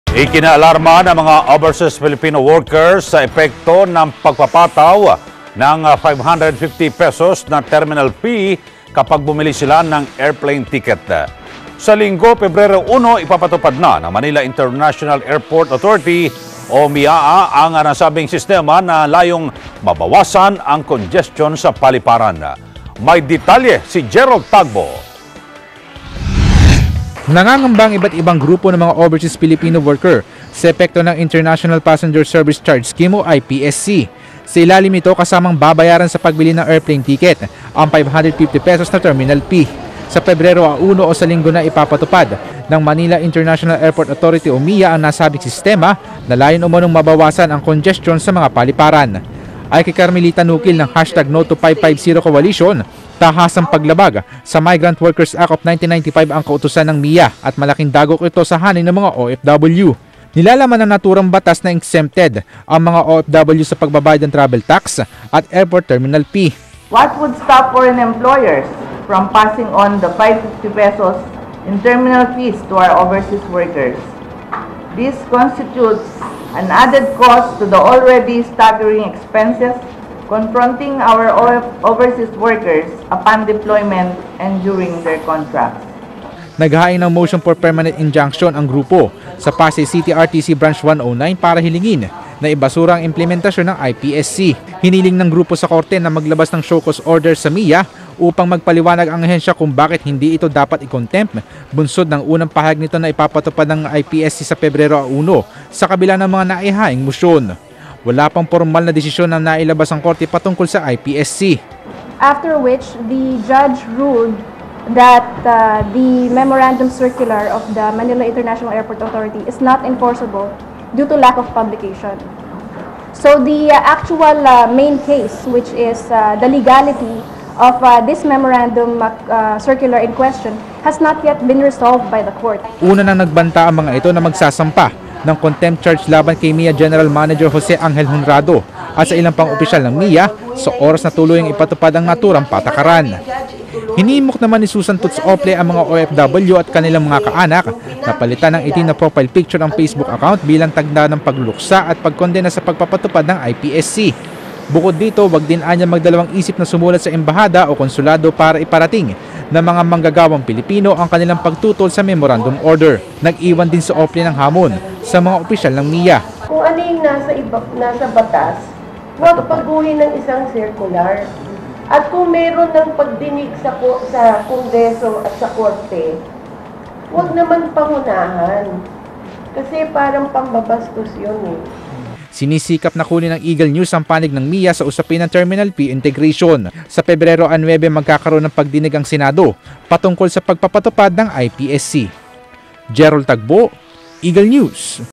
OFWs show concern regarding cargo fleet and payment of summons
Overseas Filipino workers were dismayed with the implementation of higher terminal fees.

Fast and reliable the eagle, we'll give you the latest news and important information from the leading newscasters of Eagle News Service!